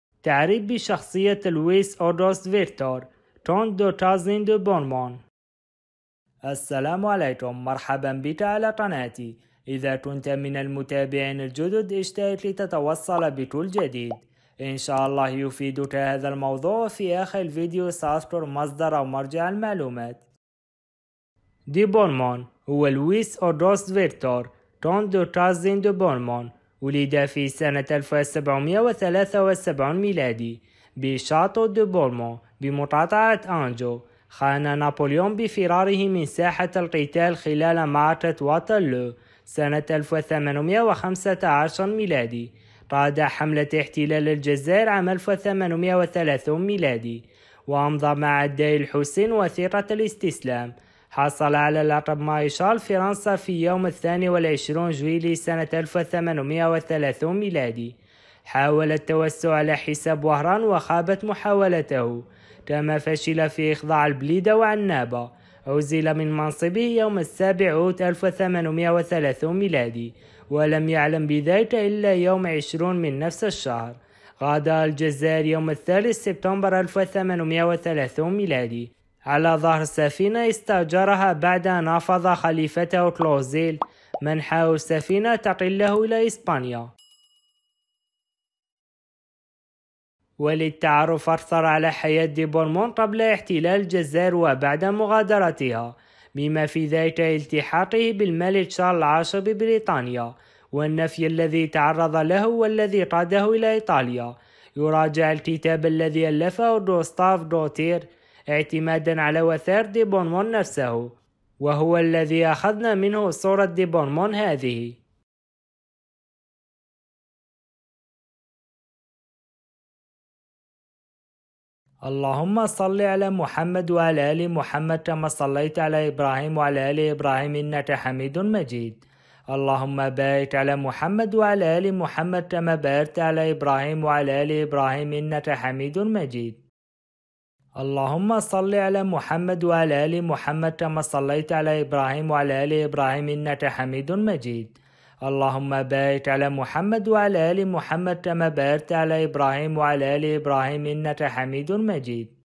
تعريف بشخصية دي بورمون Louis Auguste Victor Conte De Caine De Bourmont
الحلقة الثامنة والثمانون من سلسلة معجم المقاومة الجزائرية (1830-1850م)  بعنوان تعريف بشخصية دي بورمون Louis Auguste Victor Conte De Caine De Bourmont.

محتوى الحلقة:
00:00 عنوان الحلقة وترحيب
00:21 تعريف بشخصية دي بورمون Louis Auguste Victor Conte De Caine De Bourmont
02:01 مرجع المعلومات

ملخص السلسلة: ترجمة لشخصيات ساهمت في صناعة تاريخ الجزائر خلال عشرين سنة كاملة ابتداء من 1830م، وأدت إلى تداعيات عرفتها البلاد في الفترة اللاحقة. بالإضافة إلى جملة من الأحداث التي صاحبت المقاومة الجزائرية للاستعمار الفرنسي، ومعطيات عن بعض الأماكن التي ارتبط اسمها بالمعارك المشهورة والمدن التي كان لها شأن في المقاومة، وكذا أهم الشخصيات الفرنسية الفاعلة التي ساهمت بقوة في خدمة المشروع الاستعماري الفرنسي (معجم المقاومة الجزائرية منذ بداية الاحتلال الفرنسي حتى منتصف القرن 19 شخصيات أماكم أحداث معارك).